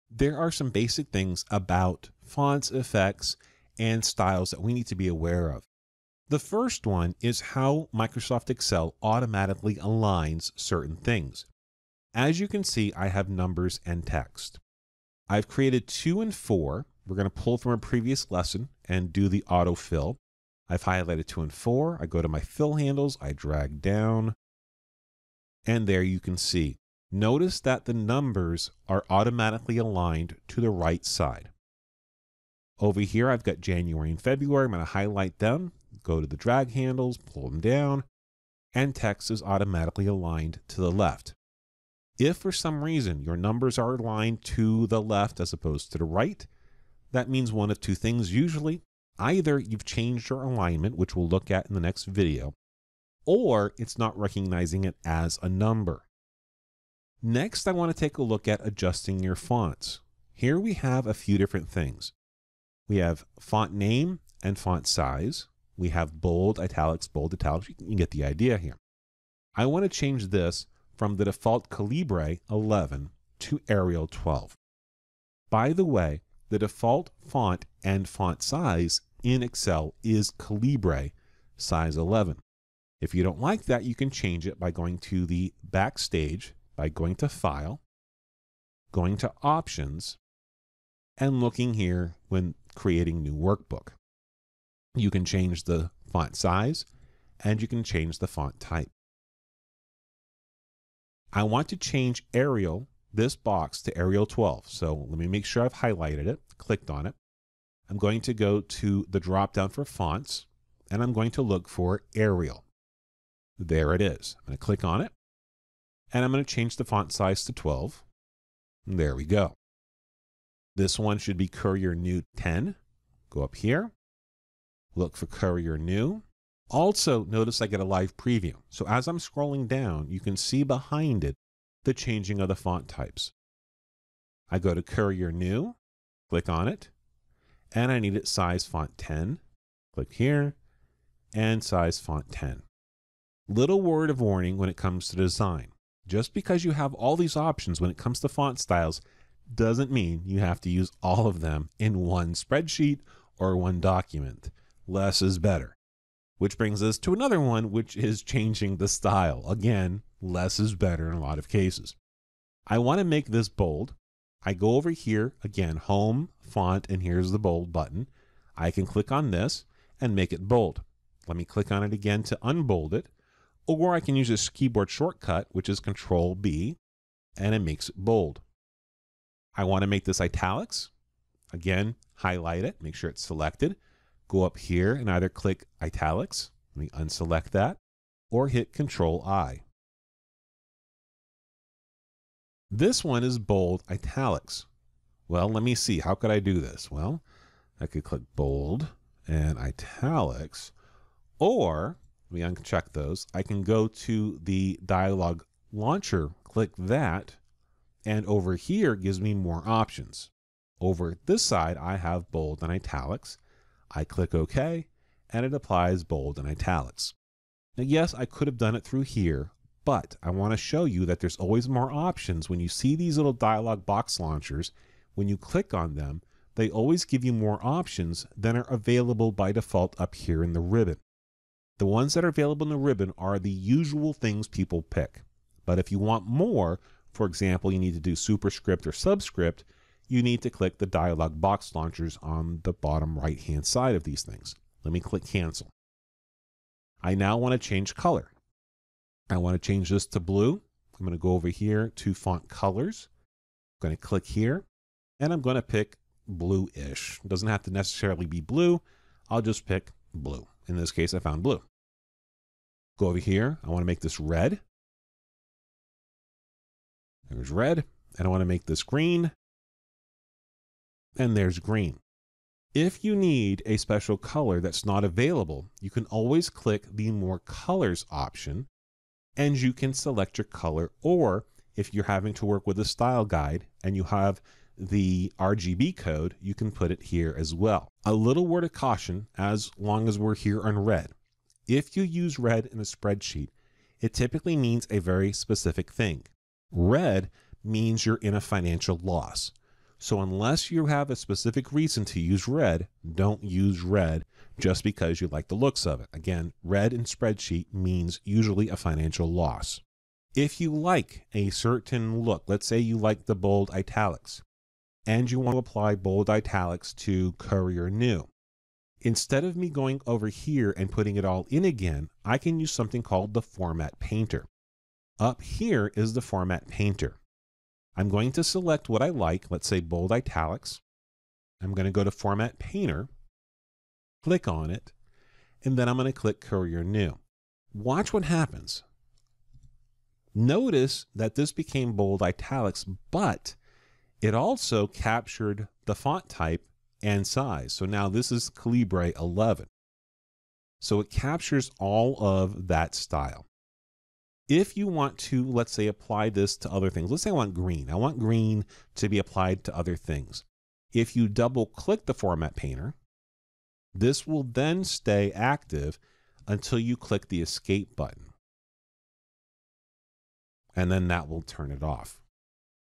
Fonts (03.05) - Microsoft Excel: From Beginner to Certified Professional
Nothing can make or break the look of your documents like font choices.

In this video, we look at the many options when it comes to formatting and selecting fonts to use in your workbooks.